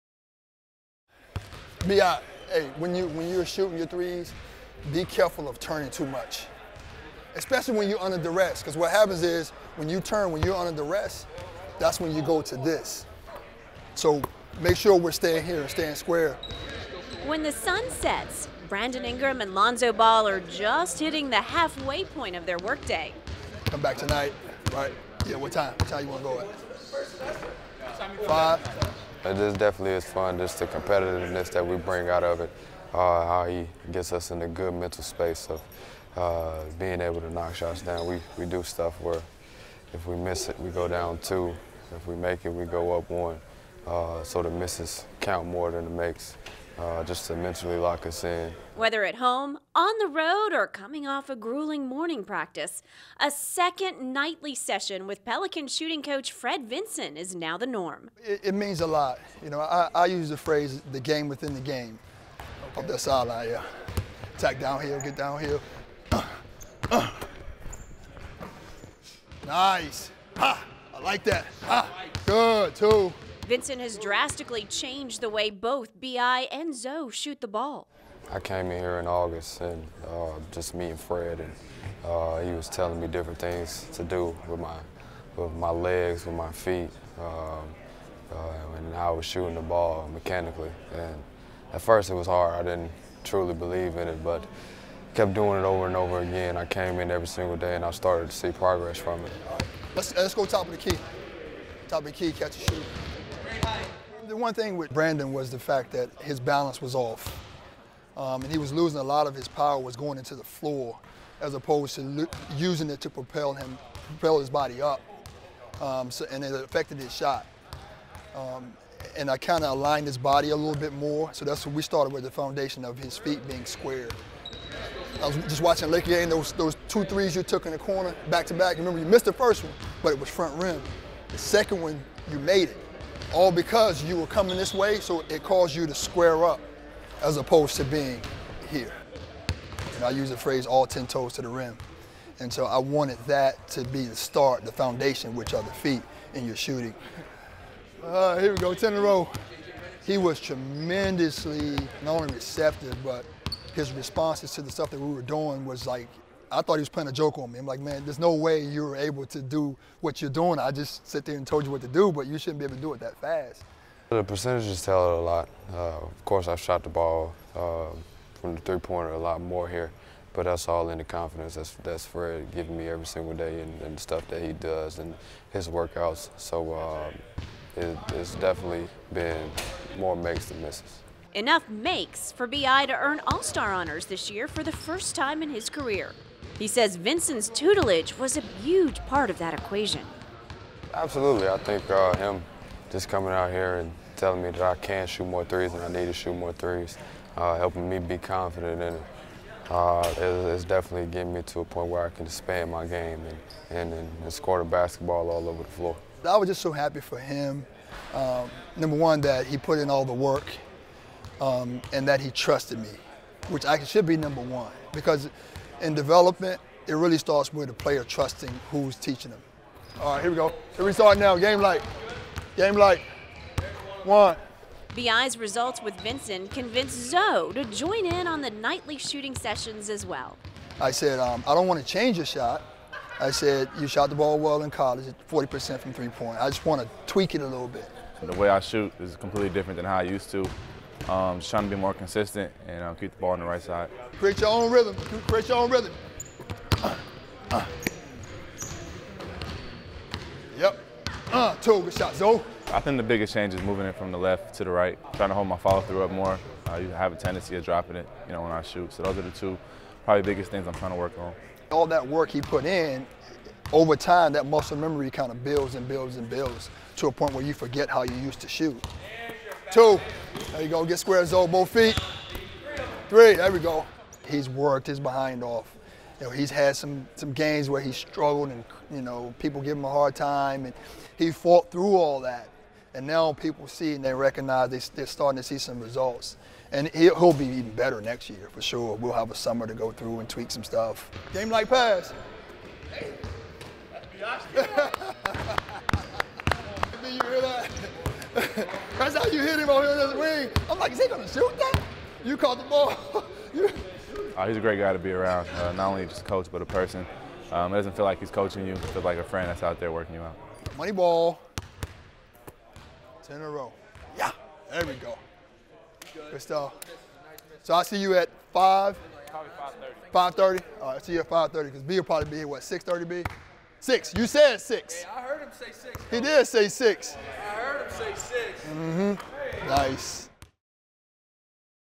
Lonzo Ball & Brandon Ingram Improve Shooting w/ Fred Vinson | Behind the Scenes
Lonzo Ball and Brandon Ingram improve their shooting mechanics, putting in extra work with Pelicans shooting coach/assistant coach Fred Vinson. Fox Sports NBA Sideline Reporter Jen Hale gives you a behind the scenes look at Pelicans practice as Fred Vinson works with BI and Lonzo to improve their jump shots.